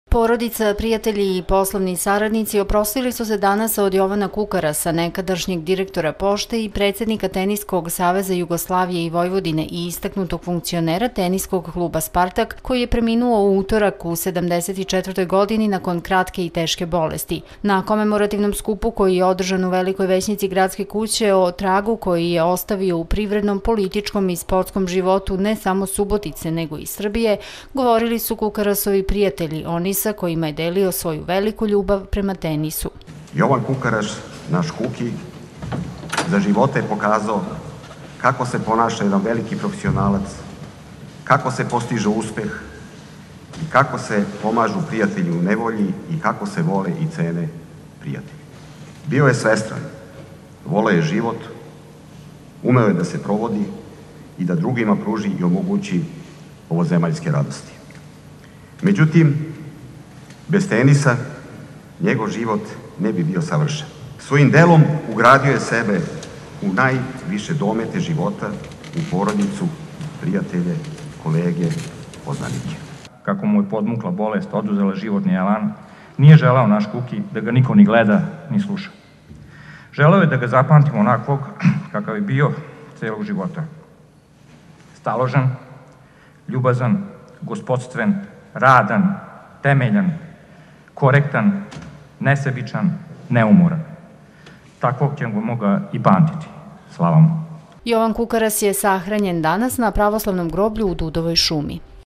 KOMEMORACIJA POVODOM SMRTI JOVANA KUKARASA
Porodica, prijatelji i poslovni saradnici, oprostili su se danas od Jovana Kukarasa, nekadašnjeg direktora Pošte i predsednika Teniskog saveza Jugoslavije i Vojvodine i istaknutog funkcionera Teniskog kluba Spartak, koji je preminuo u utorak u 74. godini nakon kratke i teške bolesti. Na komemorativnom skupu koji je održan u Velikoj većnici Gradske kuće, o tragu koji je ostavio u privrednom, političkom i sportskom životu ne samo Subotice, nego i Srbije, govorili su Kukarasovi prijatelji, oni sa kojima je delio svoju veliku ljubav,  prema tenisu . Jovan Kukaras je sahranjen danas na Pravoslavnom groblju u Dudovoj šumi.